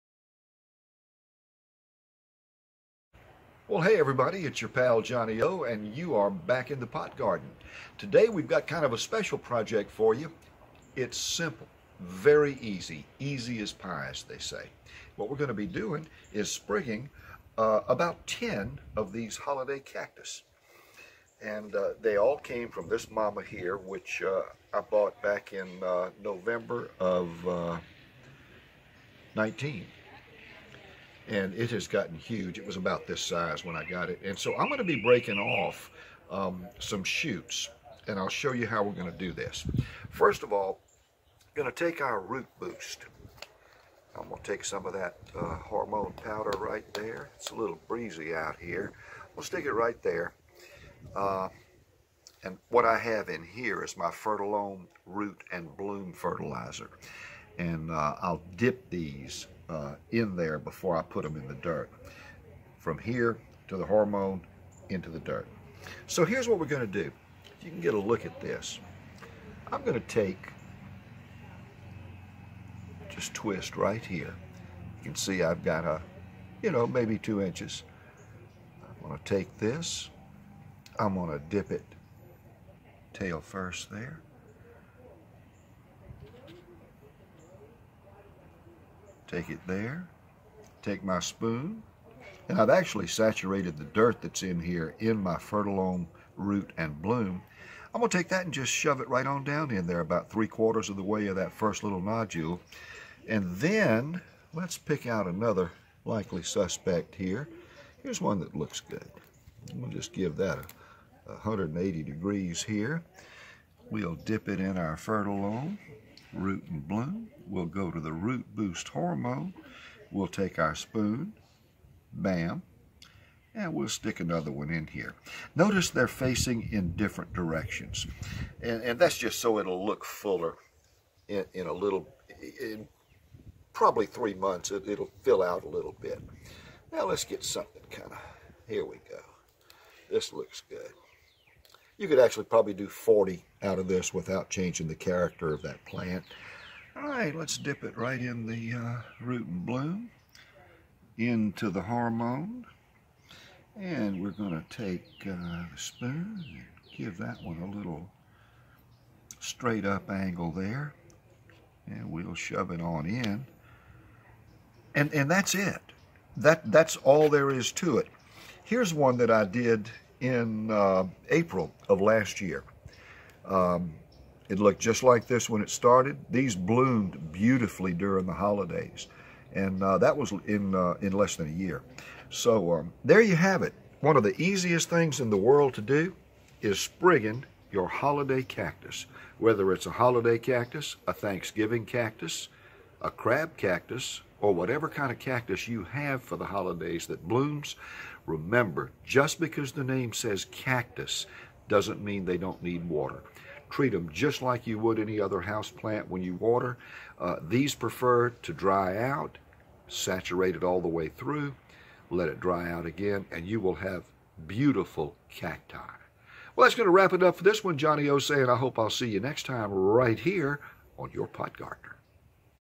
THE CRAB CACTUS
Step by step guide to sprigging your cacti.  Follow-up to a piece I did in January '21.